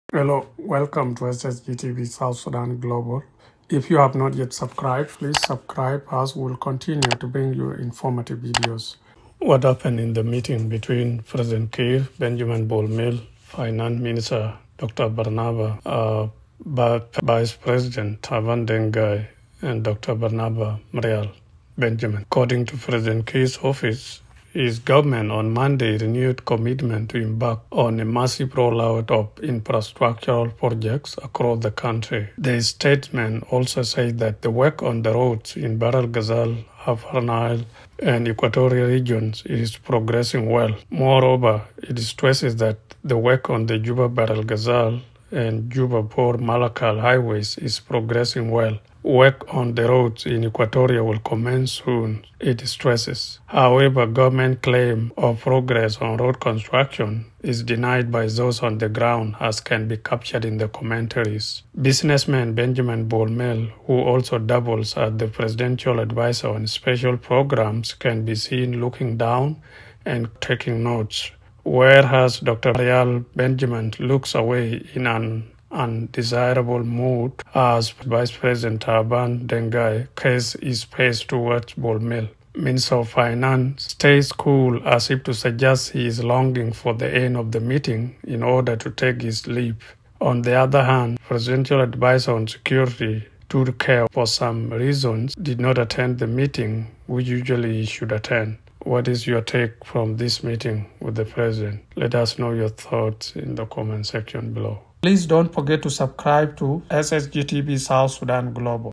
What Happened in the Meeting Between President Kiir, Bol Mel, MoFP Dr Bak, VP Taban? (Issue 59)
What happened in the meeting between President Kiir, Business mogul Benjamin Bol Mel, Finance Minister Dr Barnaba Bak, VP Taban Deng Gai and Presidential Affairs Minister Dr Barnaba Marial Benjamin? According to President Kiir’s office, his Government on Monday renewed commitment to embark on a “massive rollout of infrastructural projects” across the country.
The statement also says that the work on the roads in Bahr el Ghazal, Upper Nile and Equatoria regions is progressing well. Moreover, it stresses that the work on the Juba- Bahr el Ghazal and Juba- Bor-Malakal Highways is progressing well. “Work on the roads in Equatoria will commence soon”, it stresses. However, government claim of progress on road construction, is denied by those on the ground as can be captured in the commentaries.
Businessman, Benjamin Bol Mel, who also doubles as the presidential advisor on Special Projects can be seen looking down and kinda taking notes. Whereas Dr. Marial Benjamin looks away in an undesirable mood, as VP Taban casts his face towards Bol Mel. Minister of finance minister stays cool, as if to suggest he is longing for the end of the meeting in order to take his leave. On the other hand, Presidential advisor on security Tut-Kew, for some reasons, did not attend the meeting, which usually he should attend.
What is your take away from this meeting with the President?

2023 | SSGTV South Sudan Global